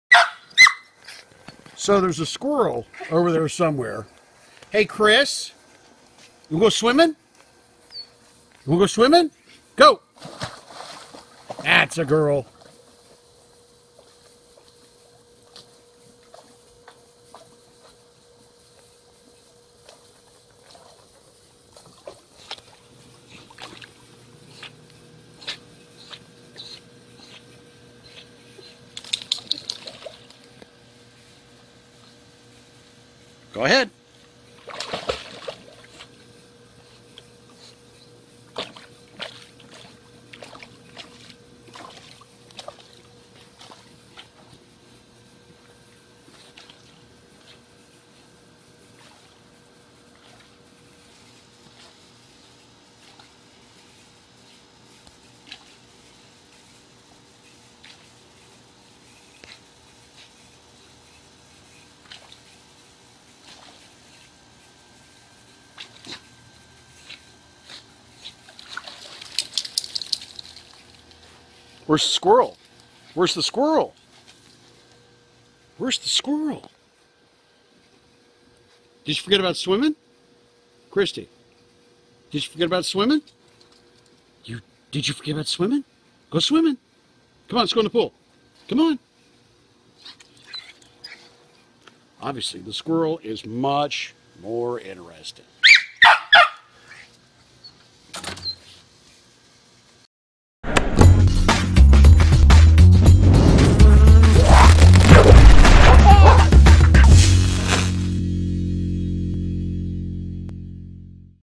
Tough decision...The squirrel or the pool?
Torn between her too loves, Christy is in a quandary.